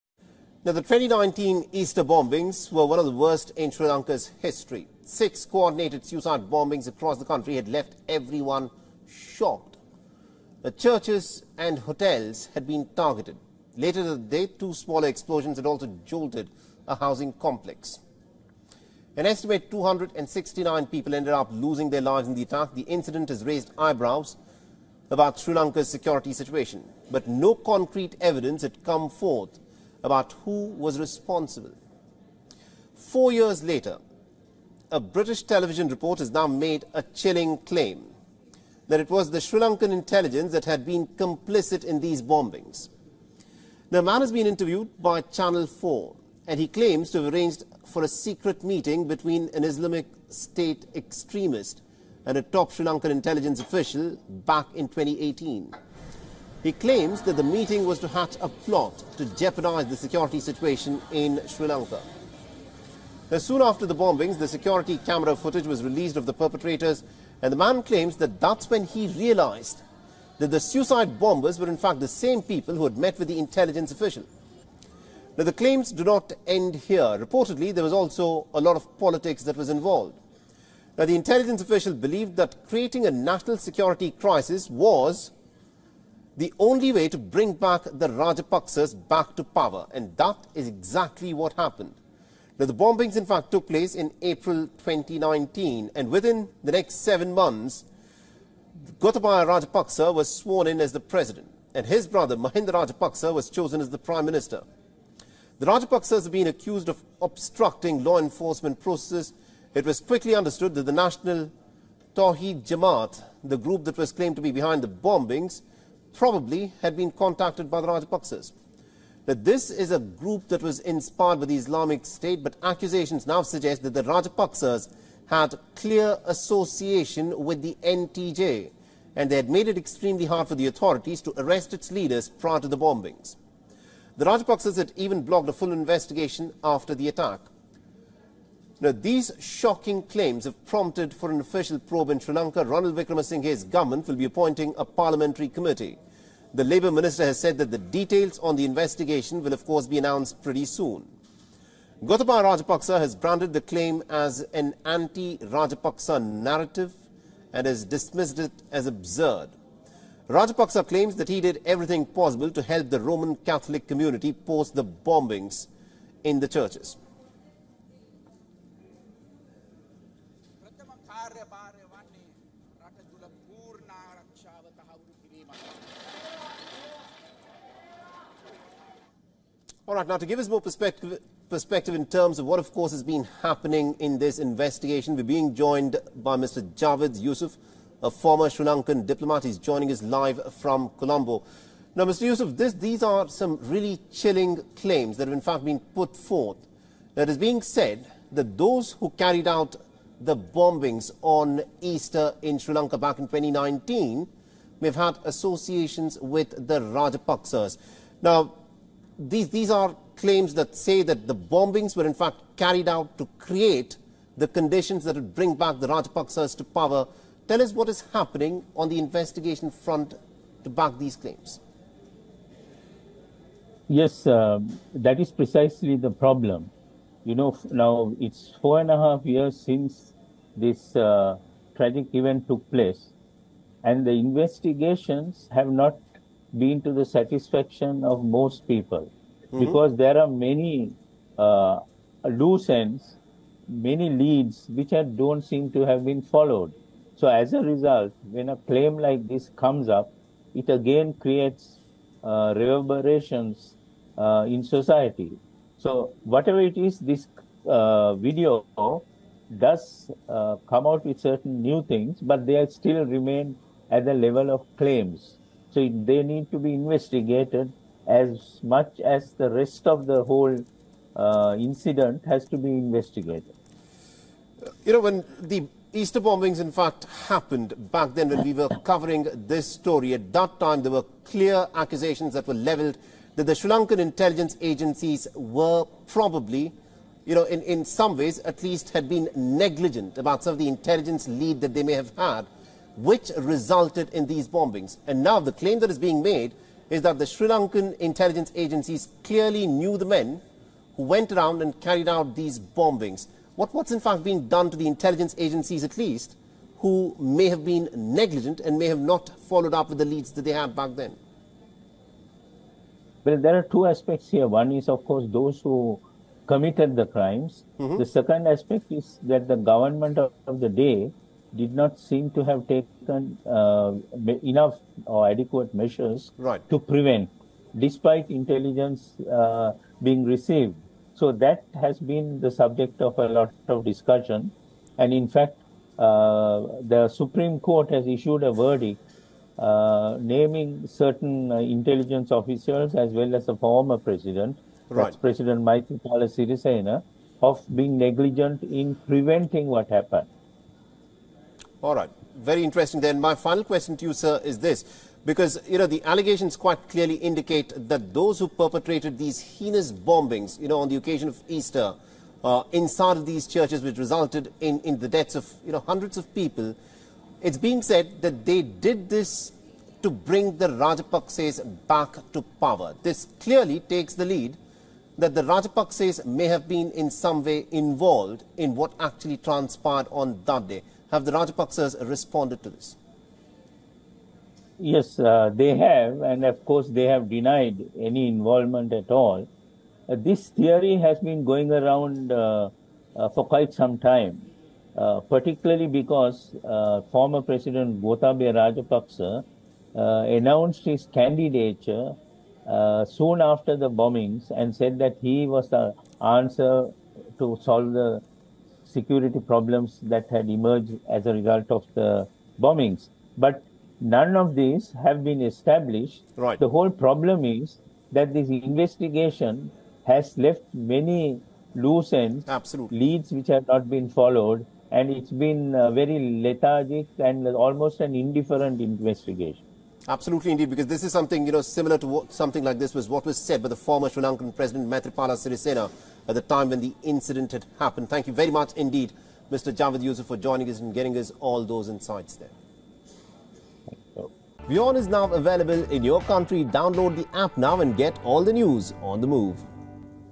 Sri Lankan government to probe 2019 bombings | WION Fineprint
On the 21st of April 2019, a group of Sri Lankans inspired by the Islamic State carried out six coordinated suicide bombings in churches and tourist hotels 269 people were killed in these attacks including worshipers at the Easter Sunday Services. Four years later the Wickremasinghe-led government is getting up to probe whether the state intelligence service was complicit in one of the most devastating extremist attacks in Sri Lankan history.